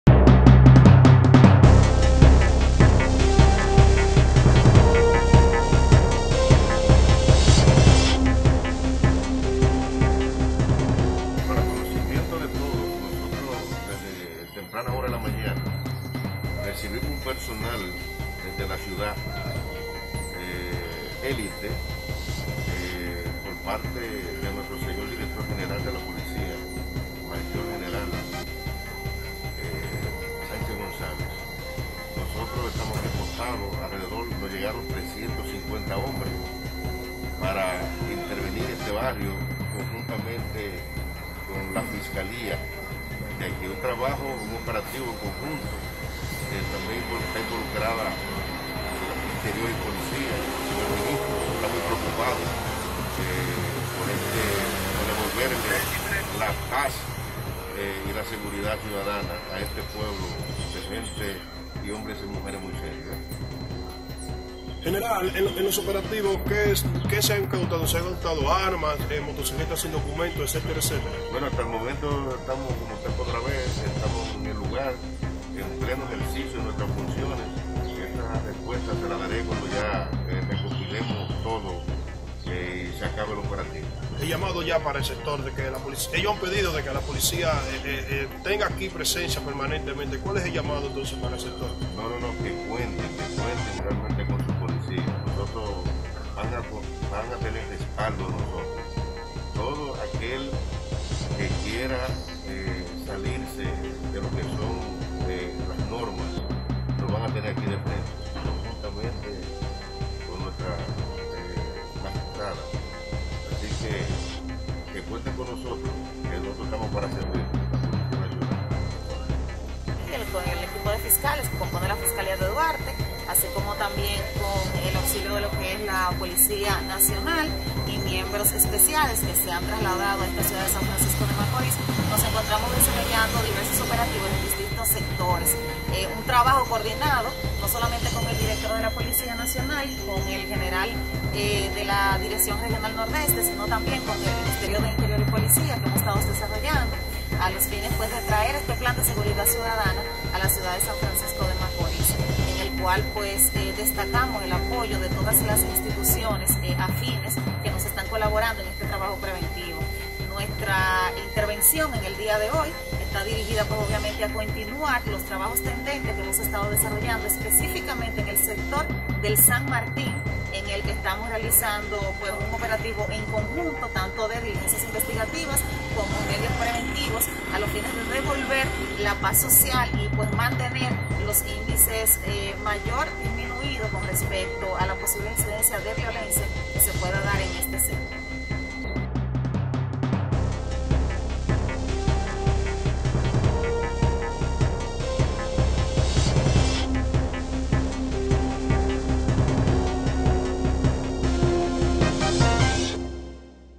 PN y Ministerio Público intervienen en el sector San Martín SFM
San Francisco De Macorís.- Mediante operativo fue intervenido el sector San Martín, en horas de la mañana del sábado 17 de julio, con la participación de miembros de la Policía Nacional y del Ministerio Público.

Según explicó el director del comando noreste de la PN, General Juan Carlos Ferreira, en este operativo participaron más de 300 agentes para intervenir en el sector, en un operativo donde también se ha implicado el Ministerio de Interior y Policía.

De su lado, la procuradora fiscal de Duarte, Smaily Rodríguez, destacó que estos operativos son un “trabajo coordinado” como parte del plan de seguridad ciudadana, y que continuarán haciendo trabajos en el populoso sector “con el fin de mantener la paz social”.